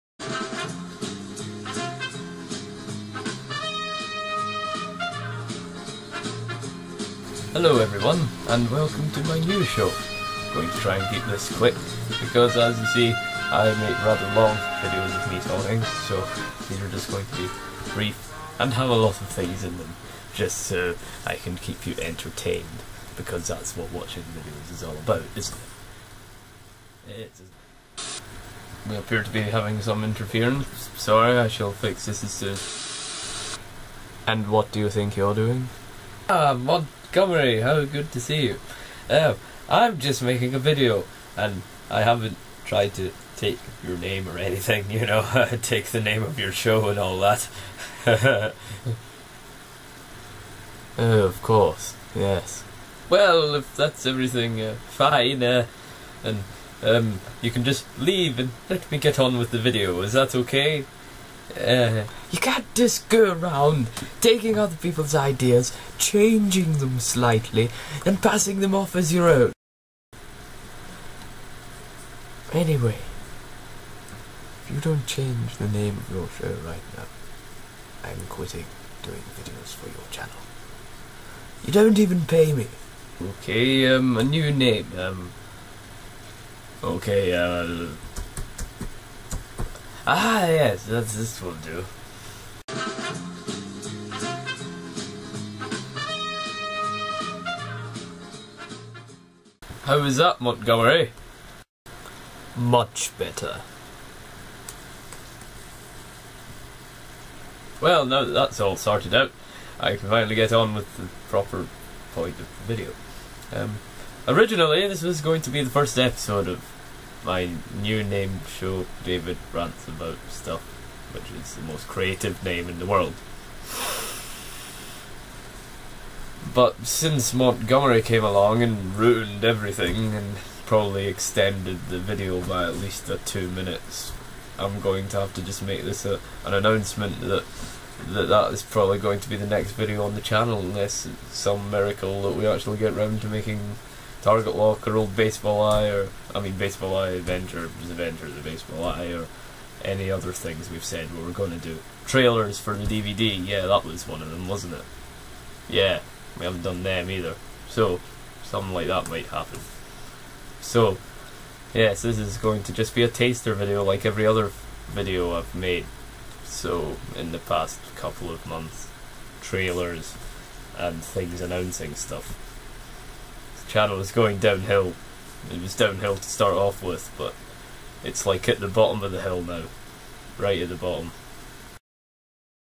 Pondering things with David
I try to make a new show but get rudely interupted